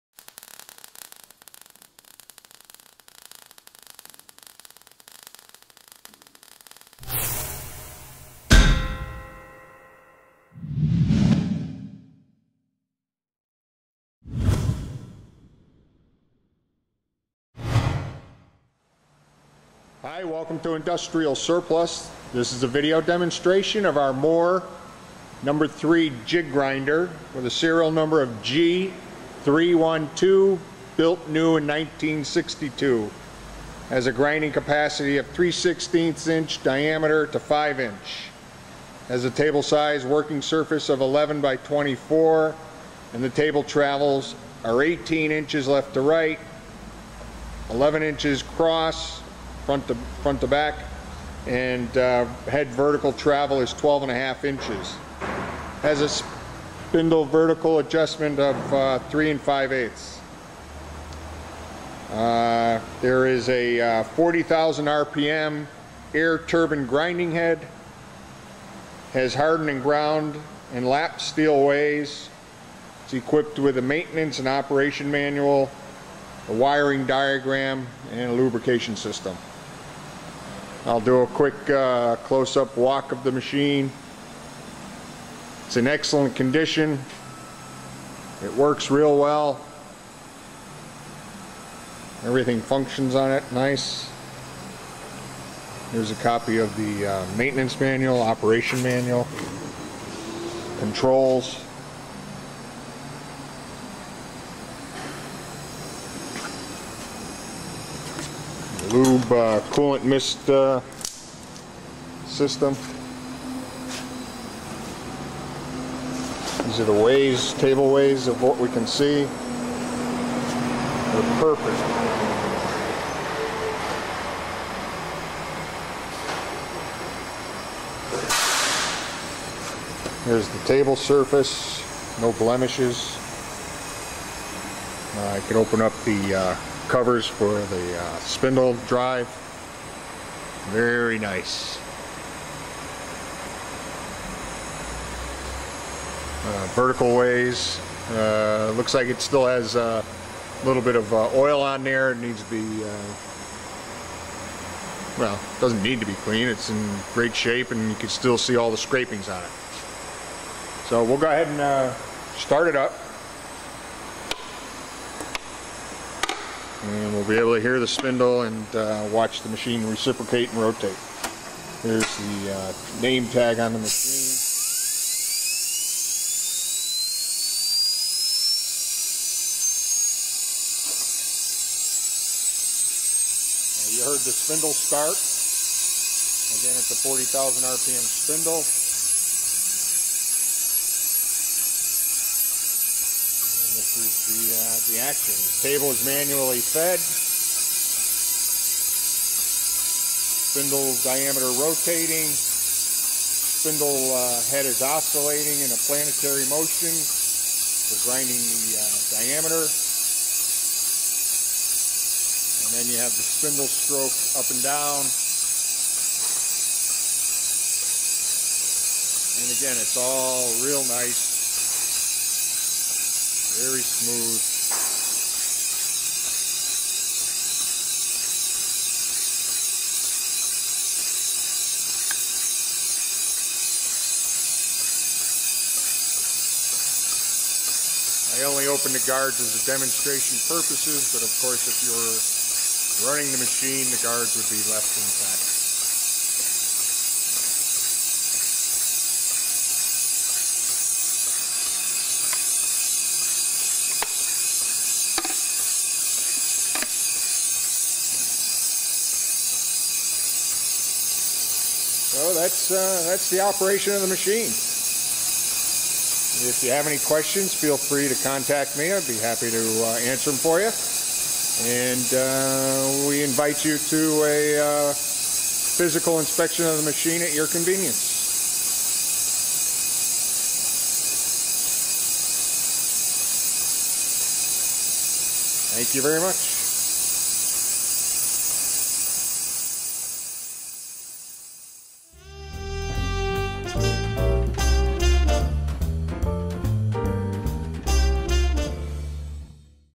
Moore #3 Jig Grinding Machine, Ref.#56-15 (SOLD)
Moore Precision Jig Grinder/ Ref.#56-15
Model No. 3
Serial No. G312     New 1962

Grinding Capacity:3/16" - 5"
Table Size:11" x 24"
Longitudinal Table Travel:18"
Cross Saddle Travel:11"
Head Vertical Travel:12-1/2"
Spindle Vertical Travel:3-5/8"
Dist. From Table Top to Wheel Collet:2" to 18"
Spindle to Column Ways:10"
Spindle to Column Below Ways:13-1/2"
Spindle Speeds, Variable:40 - 250 RPM
Spindle Feeds, Variable:4 - 240 SPM
Full Range of Grd. Wheel Speeds, Using (4) Heads:6,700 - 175,000 RPM
Grinding Wheel Speeds, with Equipped Head:40,000 RPM
Taper Adj. of Spindle:+/- 1-1/2 Deg.
Radial Offset of Grinding Spindle:Center to 1-1/8"
Overall Size of Machine:50" x 68" x 96" High
Machine Weight, Approx.:4400 Lbs.

Equipped With:
Moore L-1691, 40,000 RPM Air Turbine Grinding Head
Hardened, Ground & Lapped Steel Ways
Maintenance & Operation Manual
Wiring Diagram
Lube System
Work Light

Excellent Condition